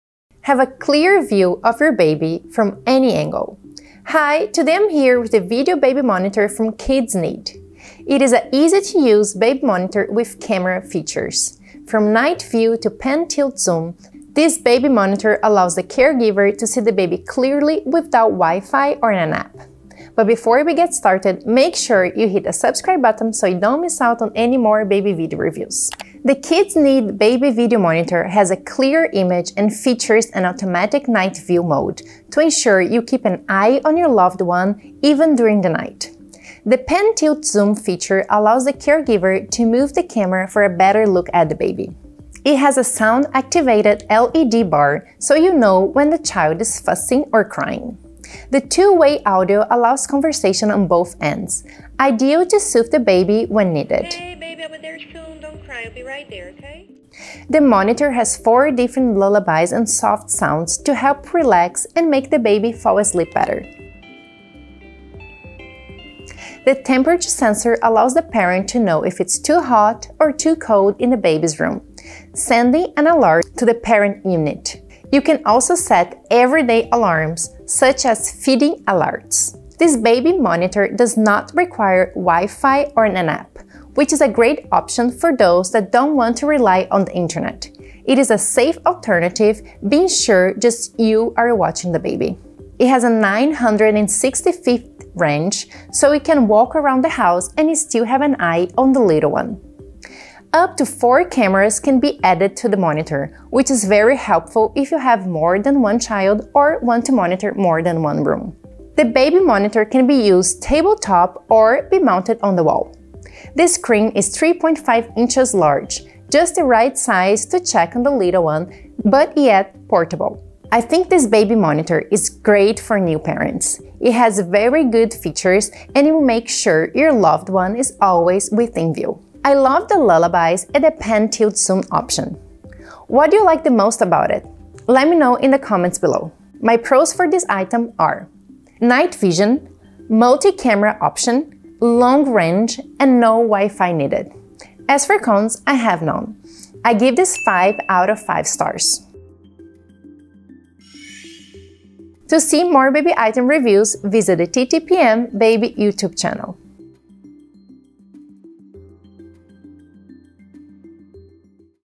Video Baby Monitor from Kidsneed Review!
Today TTPM is reviewing the Video Baby Monitor from Kidsneed. It is an easy-to-use baby monitor with camera features. From night view to pant tilt zoom, this baby monitor allows the caregiver to see the baby clearly without wifi or an app. See more in this video review. For full review and shopping info

Product Info: Have a clear view of your baby, from any angle. The Kidsneed Baby Video Monitor has a clear image and features an automatic night view mode to ensure you keep an eye on your loved one even during the night.